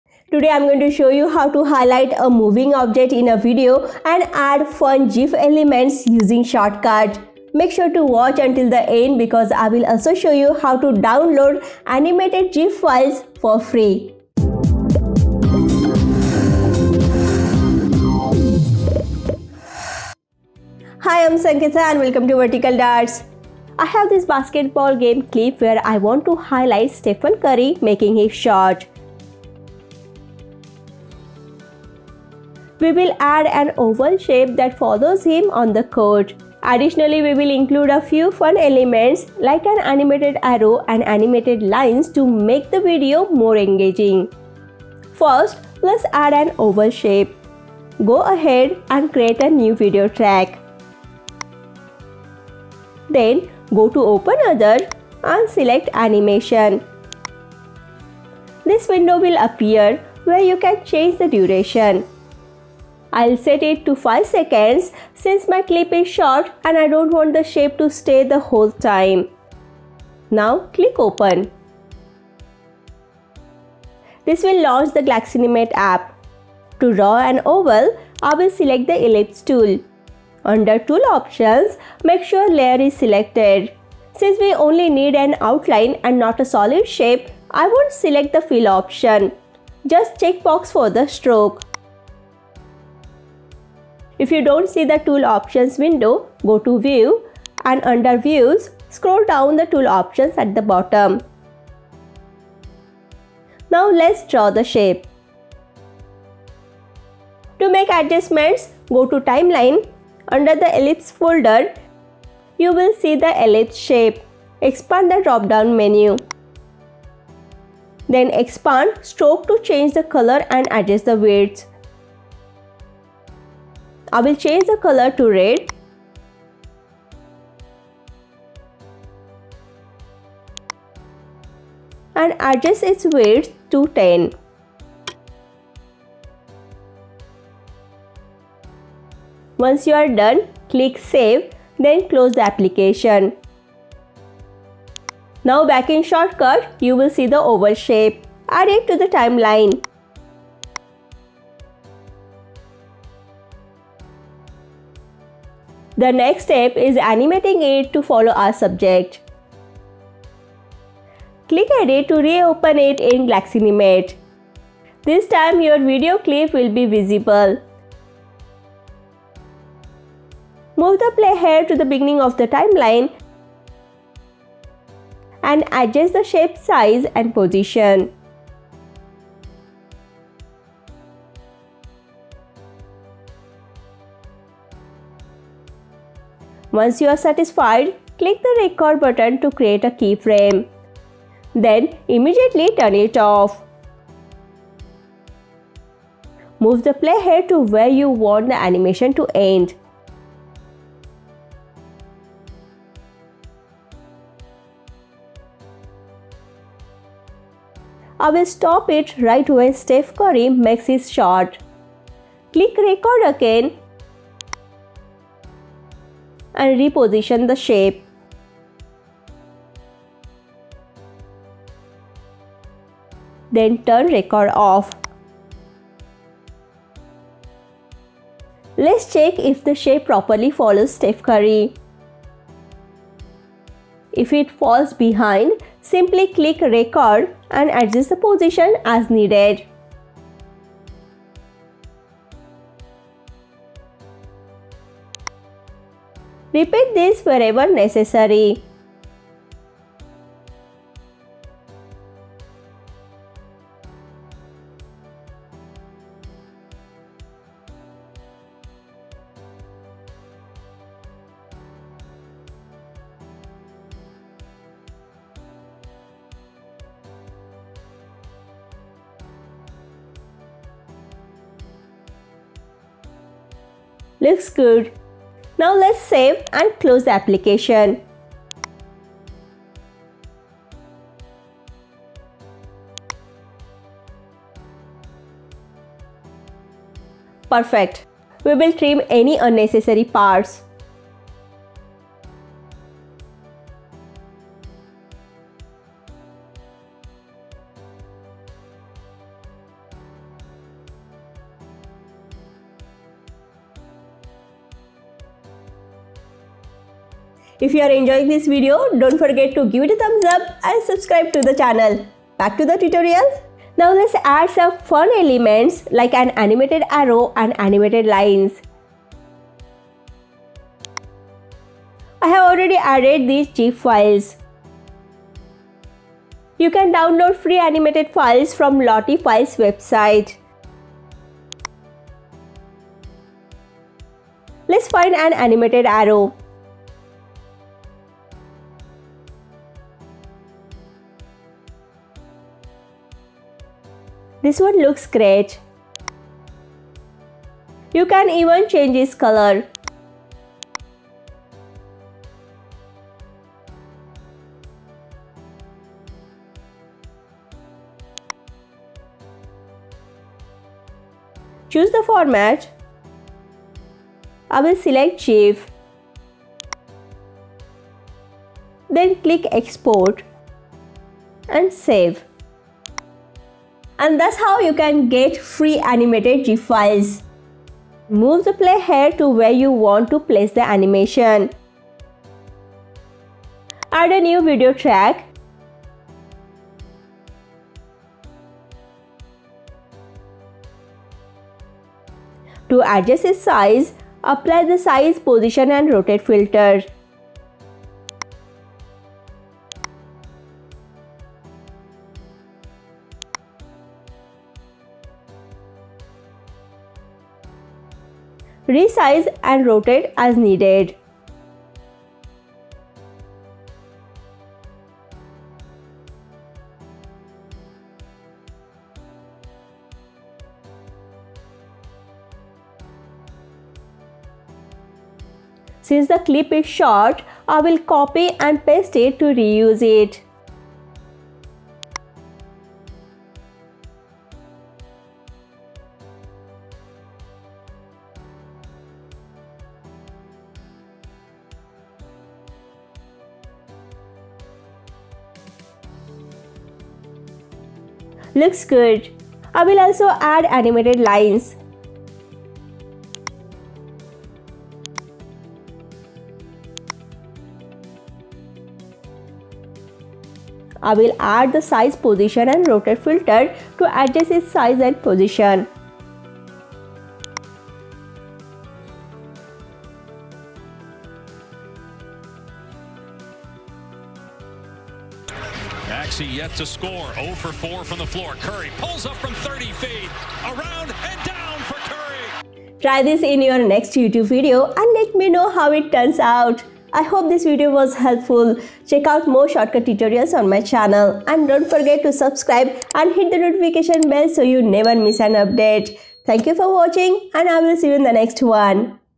How to Highlight Moving Objects & Add Fun GIFs to Videos | Shotcut Tutorial
Want to make your videos stand out? In this tutorial, I’ll show you how to highlight a moving object in a video and add animated GIF elements using Shotcut! This is perfect for sports clips, tutorials, and eye-catching edits. Plus, I’ll also show you where to download animated GIFs for free!

🔹 In This Video, You’ll Learn:
✅ How to add an oval shape to highlight a moving subject
✅ How to animate the shape to follow the object
✅ Where to download free animated GIFs
✅ How to add fun elements like arrows and lines to enhance your video

💡 Whether you’re editing sports highlights or making your videos more dynamic, this simple motion tracking effect will take your content to the next level!